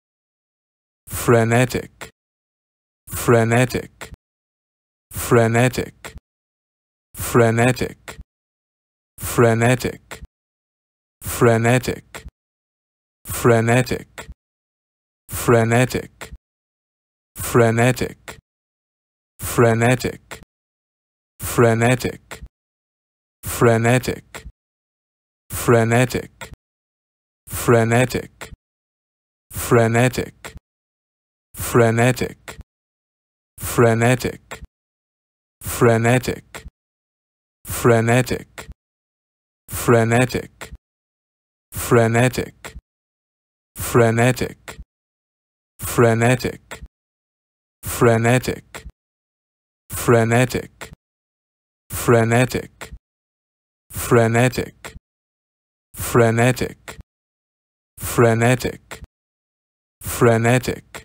How to pronounce Frenetic||How to say Frenetic||Frenetic Pronunciation||ABDictionary
Do you want to know how to pronounce Frenetic? In this video you'll learn how to say or pronounce Frenetic for free